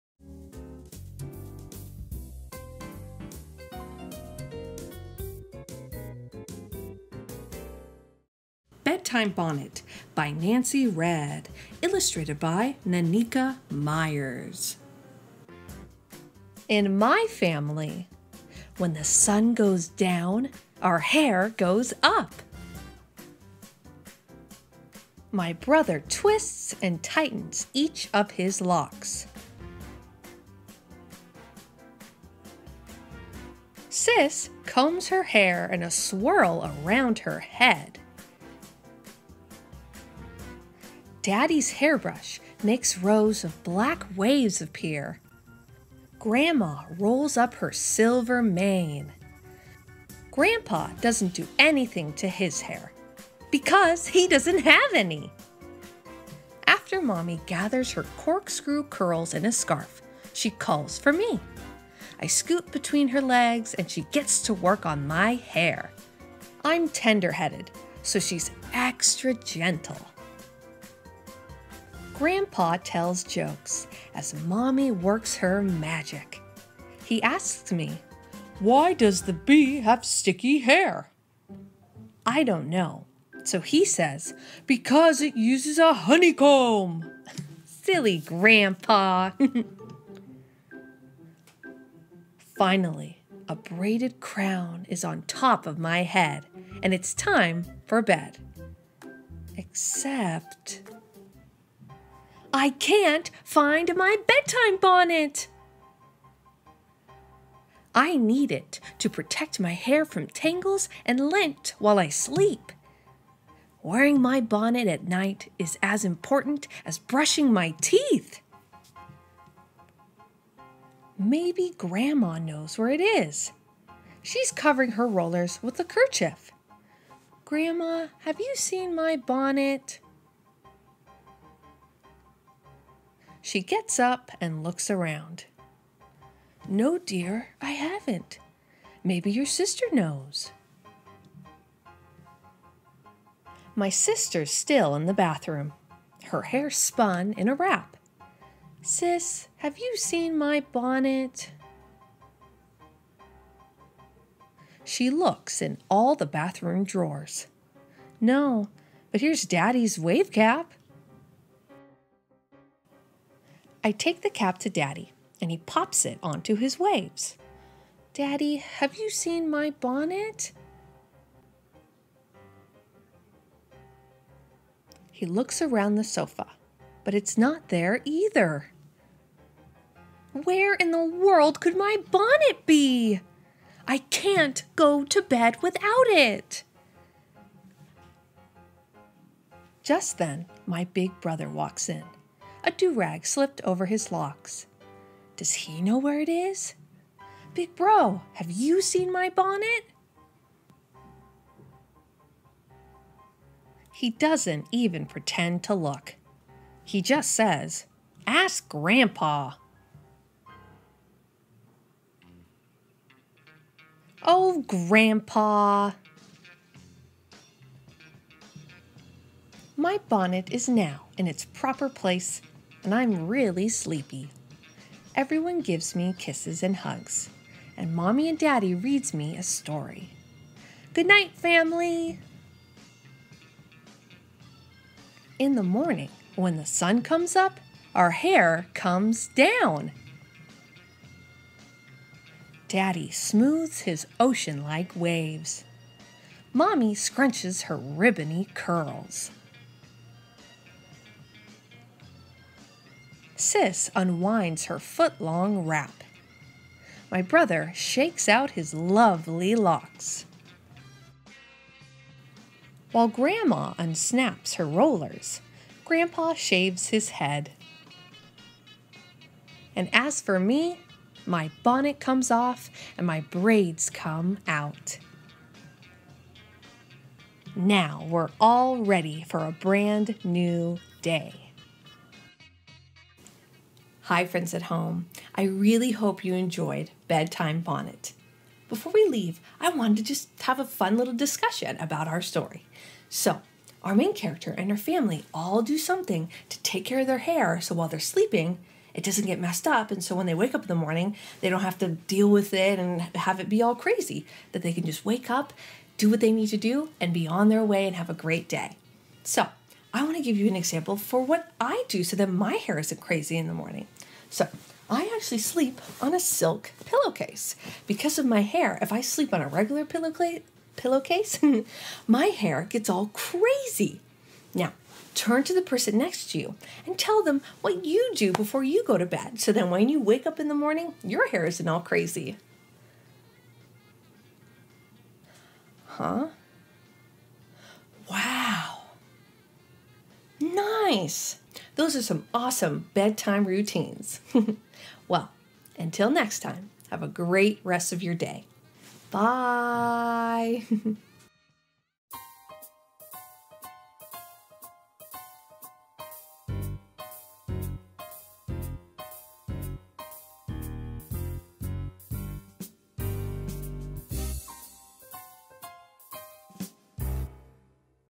Bedtime Bonnet - StoryTime with Ms. J
Bedtime Bonnet
Written by Nancy Redd
Illustrated by Nneka Myers

Grade Level: Preschool - 2nd

Facebook: StoryTime with Ms. J